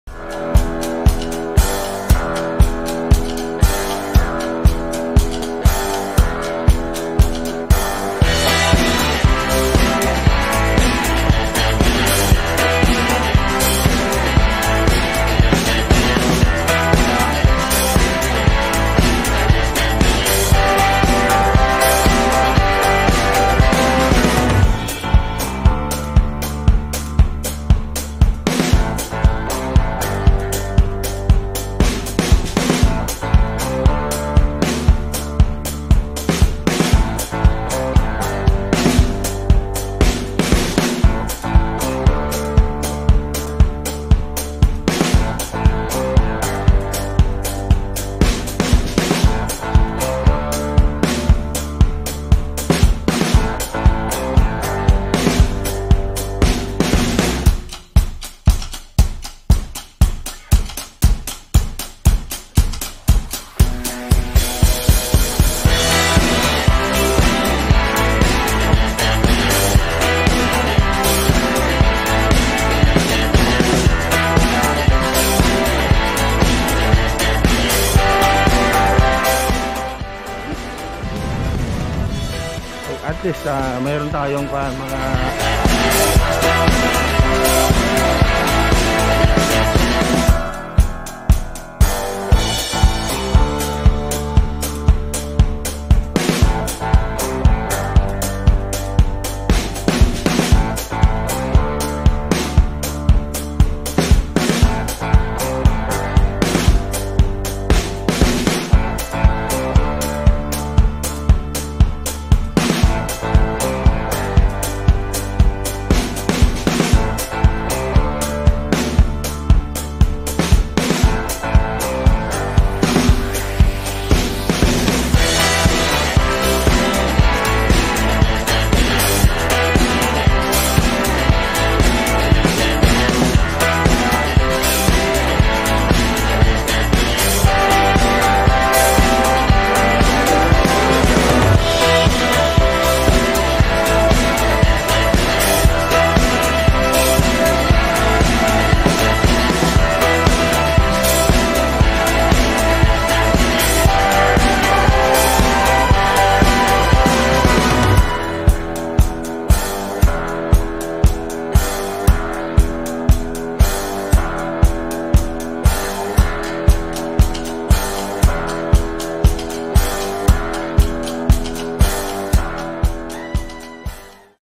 Saturday Couple Biking July 17, 2021
Biking to Wacal-PD Galima-Mapaina-Lactawan-San Luis-Brgy Osmenia-Brgy Quezon-Lee Subd-Back home